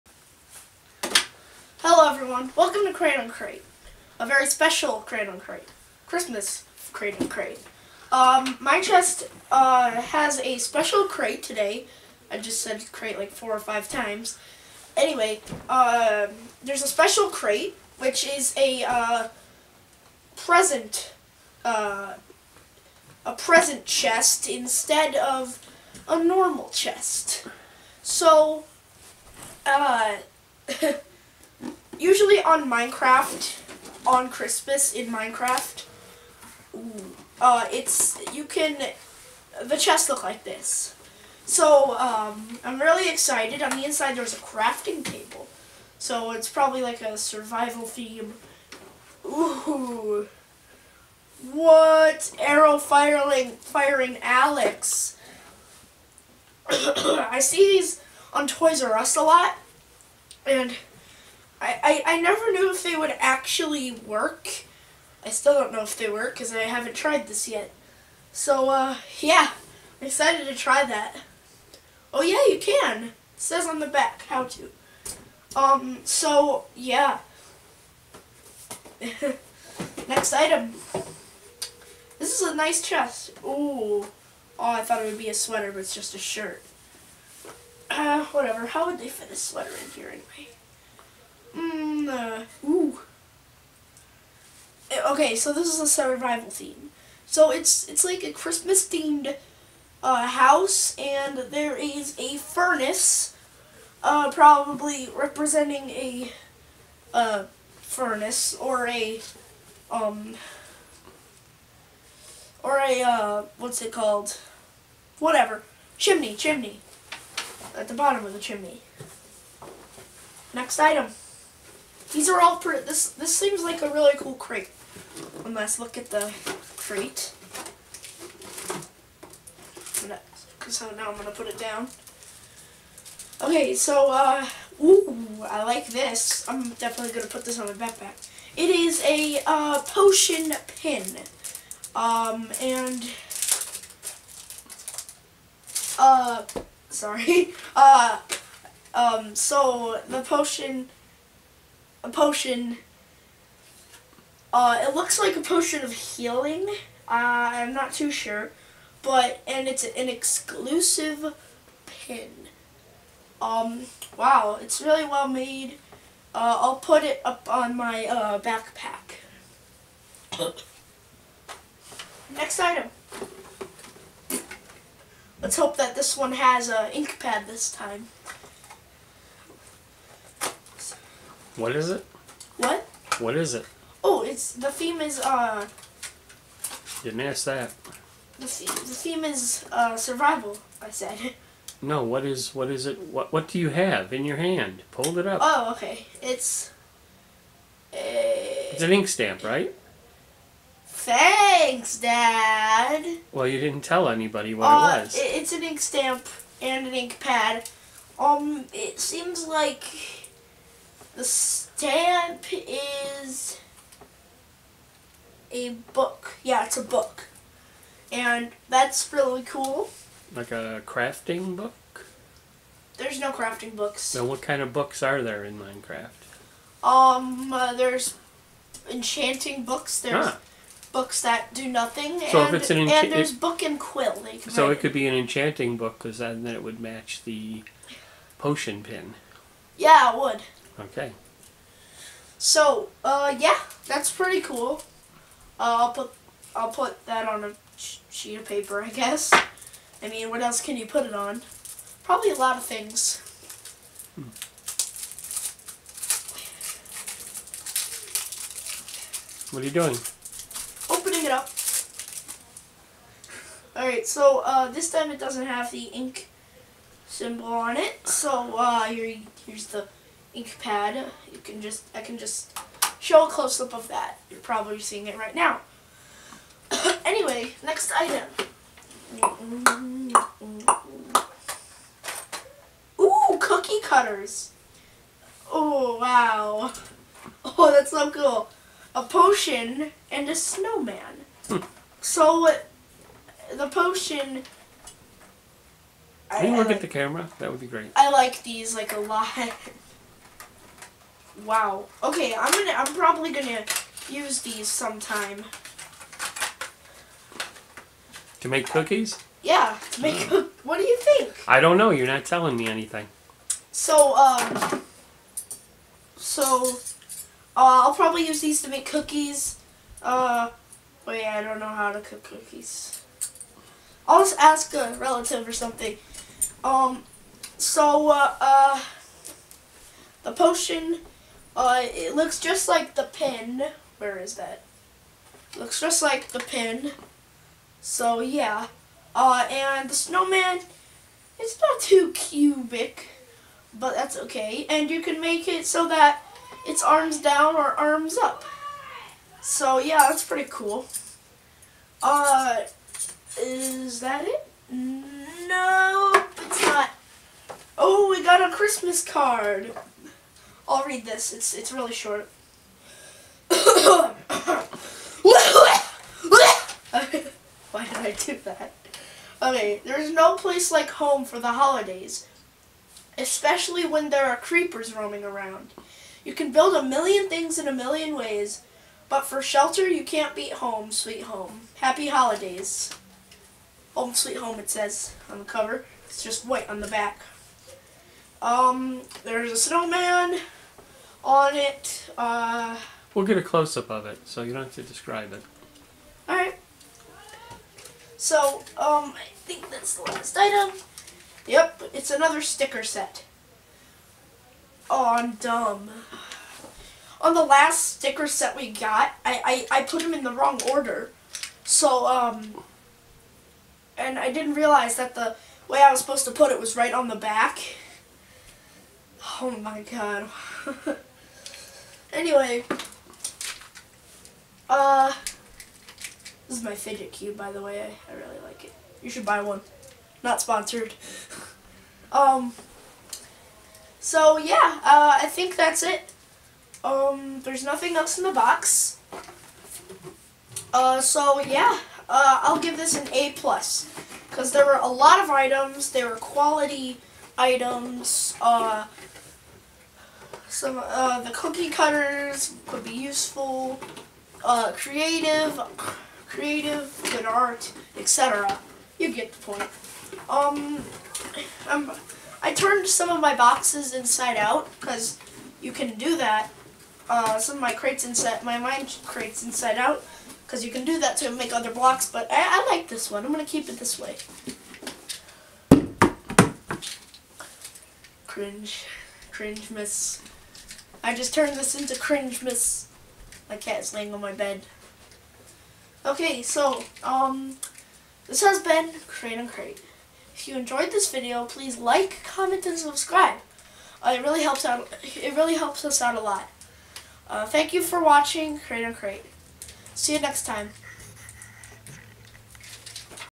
Special Mine Craft Mine Chest Christmas Box 2016 UNWRAPPED!
Special Christmas Mine Craft Mine Chest filled with all sorts of awesome Mine Craftian swag. Worth your while? Find out!